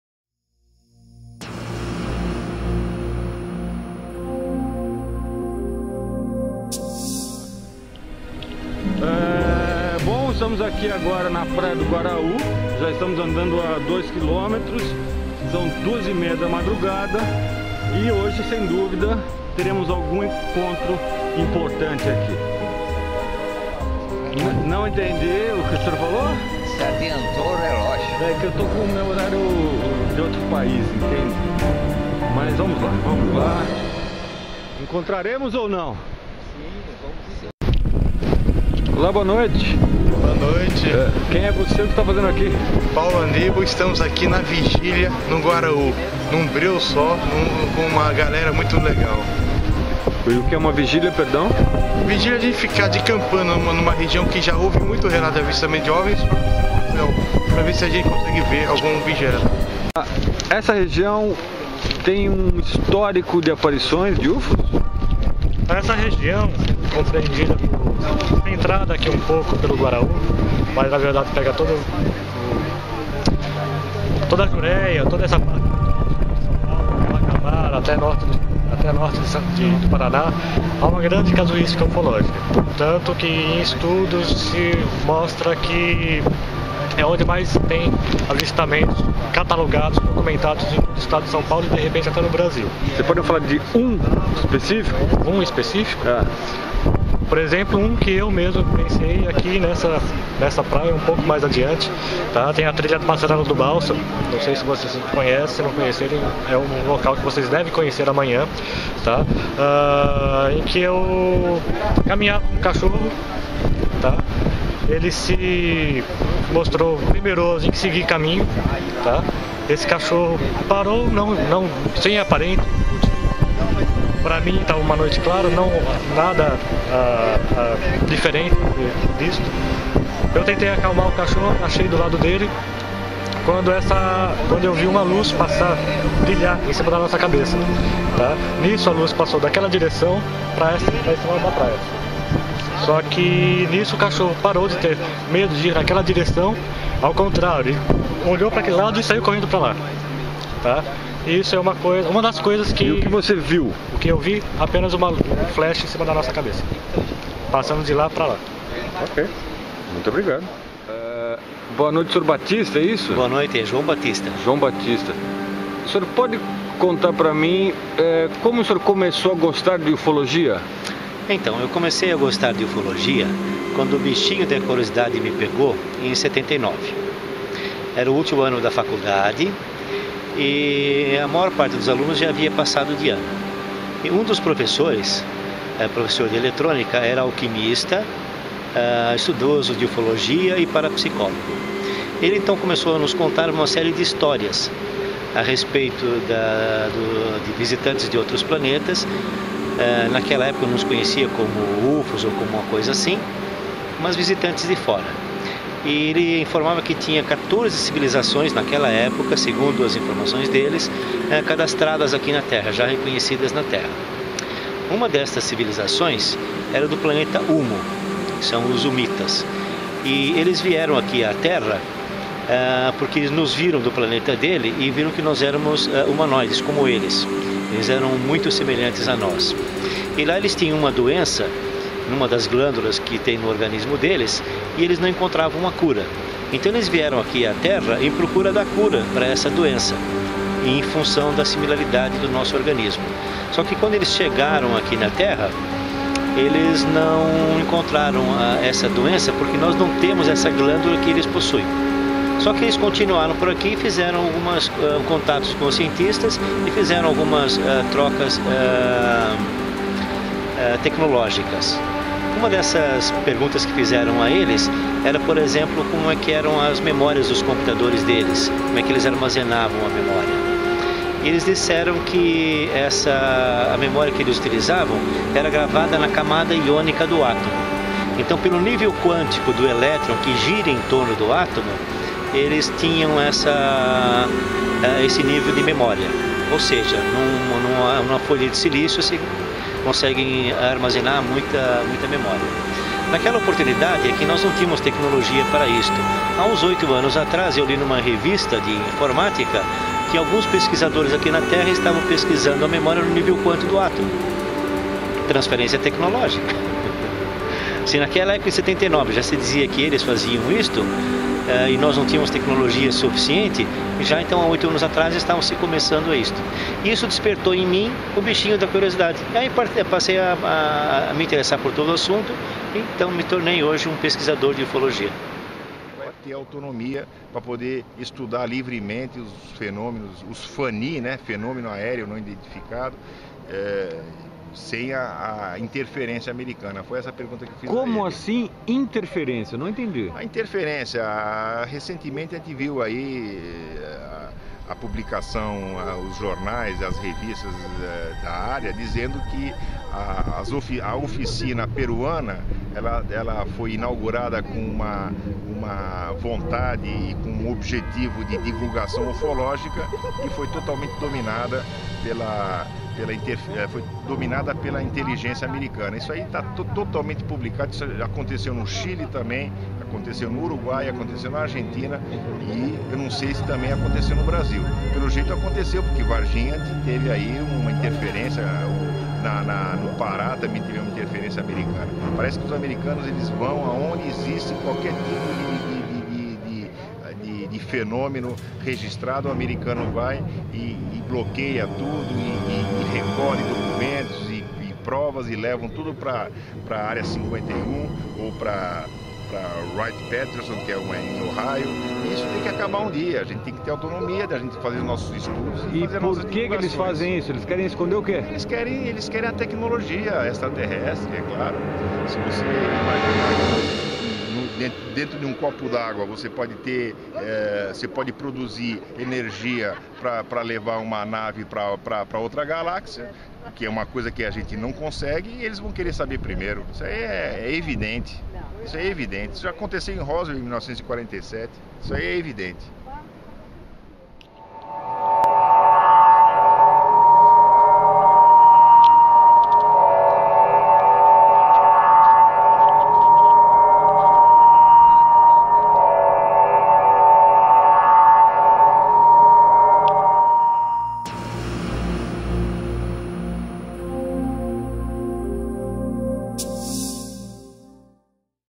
Contato Alien -Caçadores de UFO (Expedição Vigília Ufológica - Peruíbe/ Brasil) Fatos Reais
BRASIL 2012 - Uma surpreendente expedição a procura de ovnis e contatos extraterrestres. Relatos reais. Juréia/ Brasil 2012